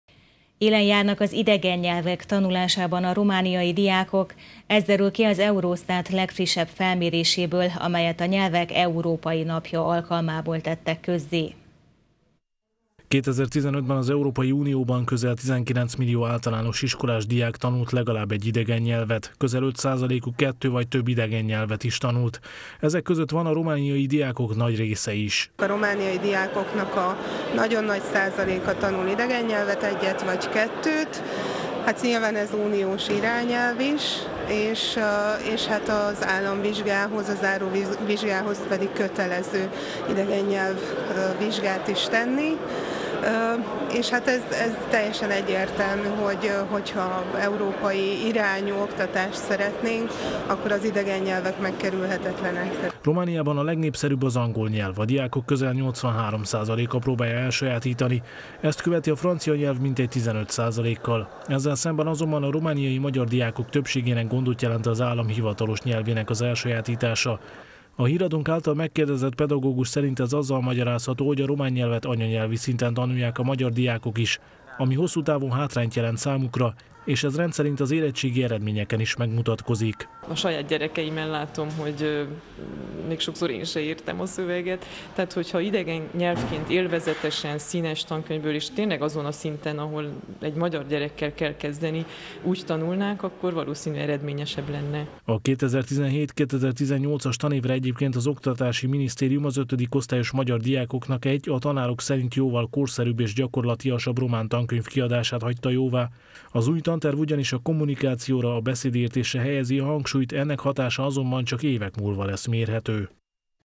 Idegennyelvtudásból jeles – Erdélyi Magyar Televízió
Élen járnak az idegennyelvek tanulásában a romániai diákok. Ez derül ki az Eurostat legfrissebb felméréséből, amelyet a Nyelvek Európai Napja alkalmából tettek közzé.